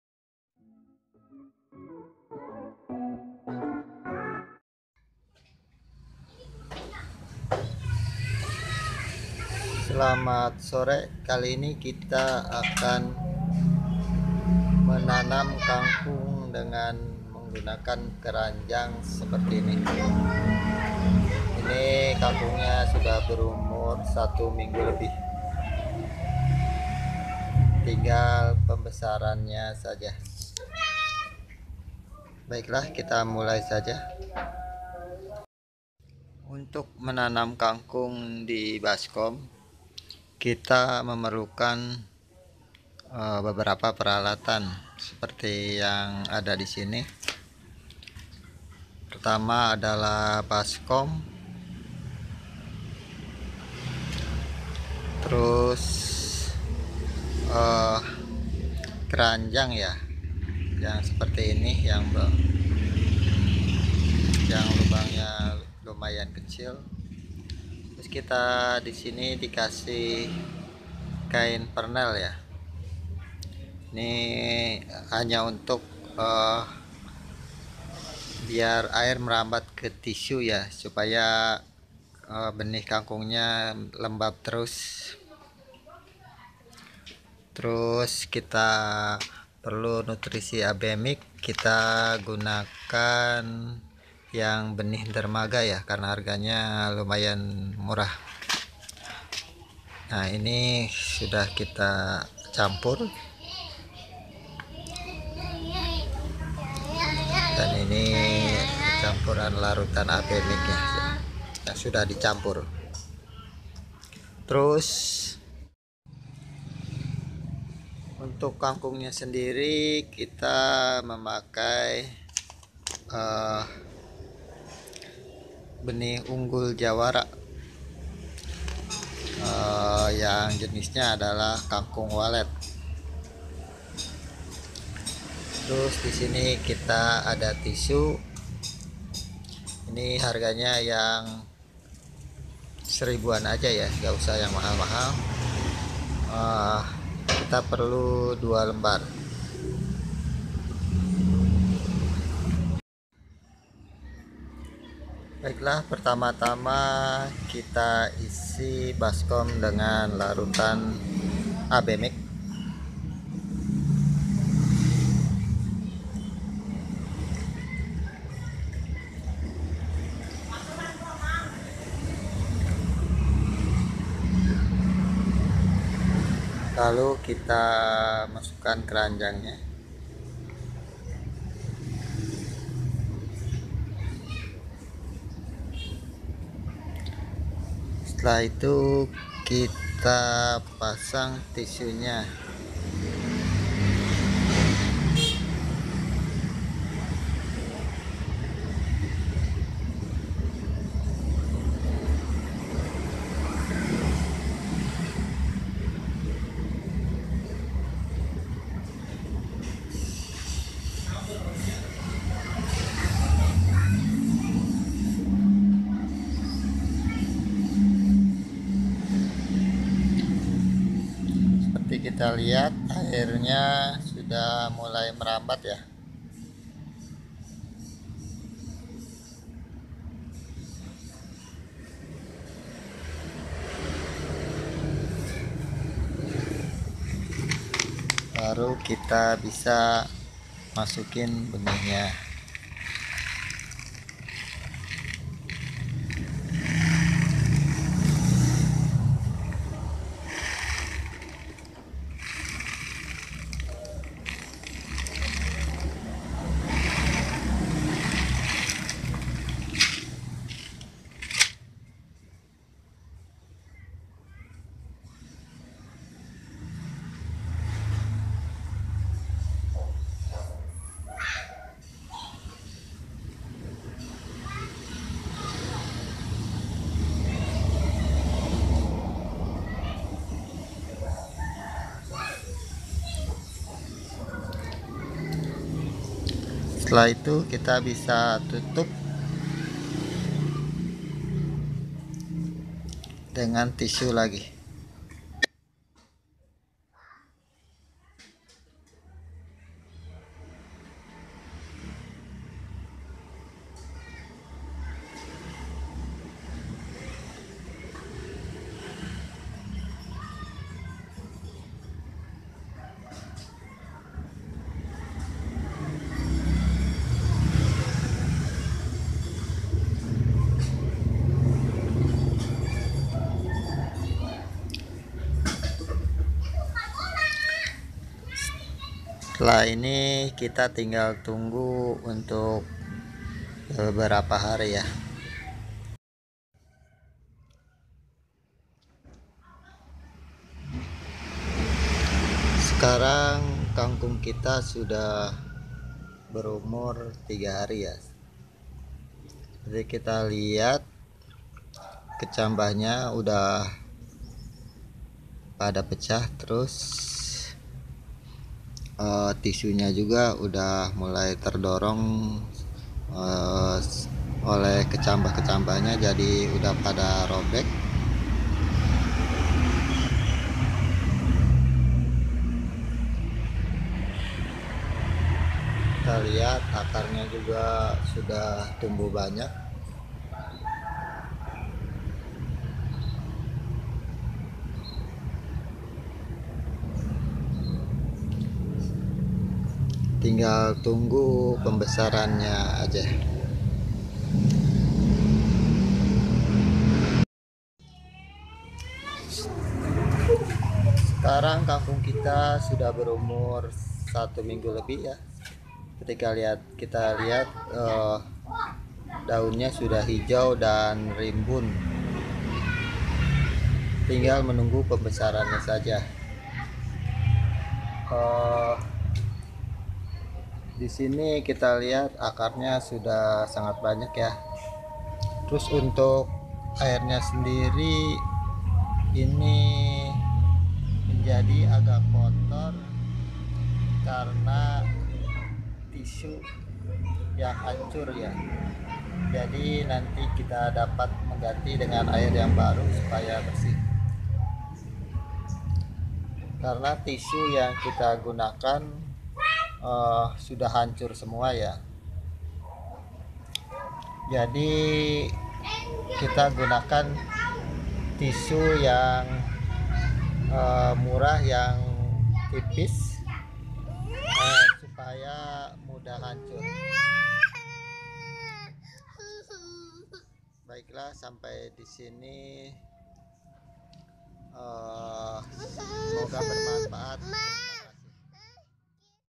Cara menanam kangkung di keranjang menggunakan tissu
Cara menanam kangkong di keranjang menggunakan tissue, sangat sederhana dan mudah.